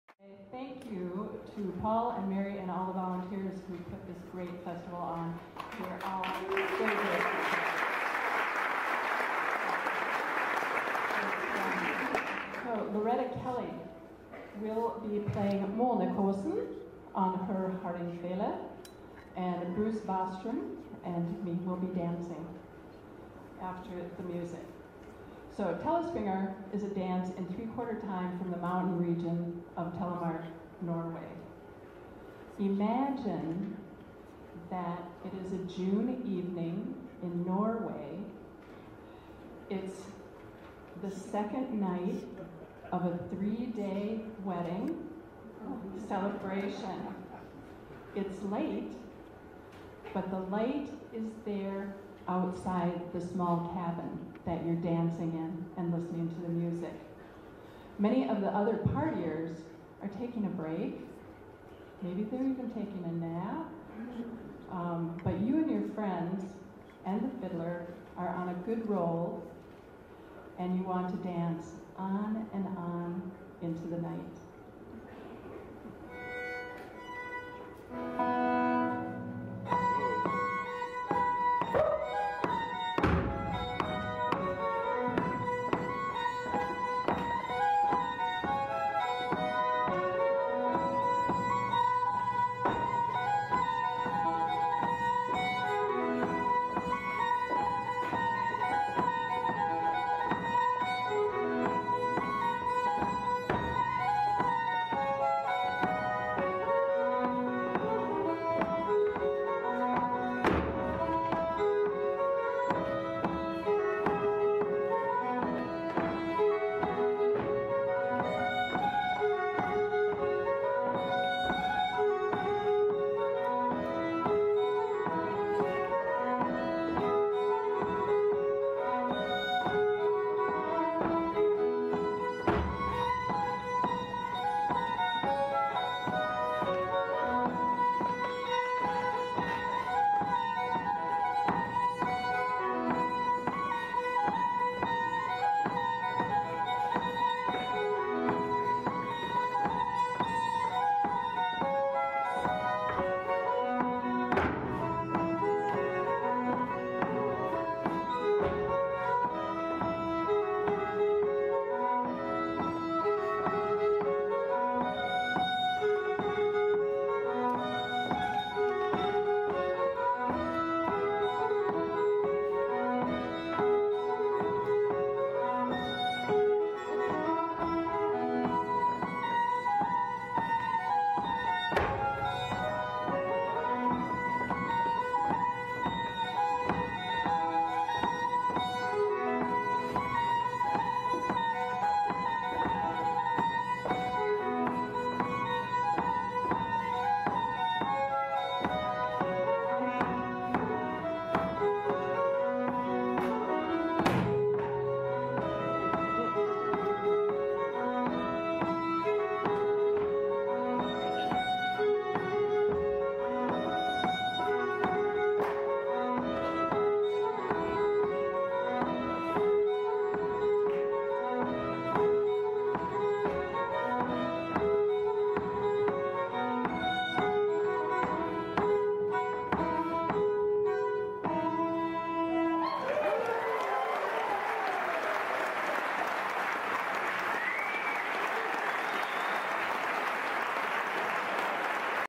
Telespringar - Loretta Kelley, Carol Sersland and Bruce Bostrom
Nisswa-stämman Friday night concert featured several dance demos.. this one features the telespringar, a traditional dance from Telemark, Norway. Loretta Kelley, hardingfele, Carol Sersland and Bruce Bostrom, dancers. Video produced and provided by Eric Jaakkola of Brooklyn Park, Mn. Thank you Eric!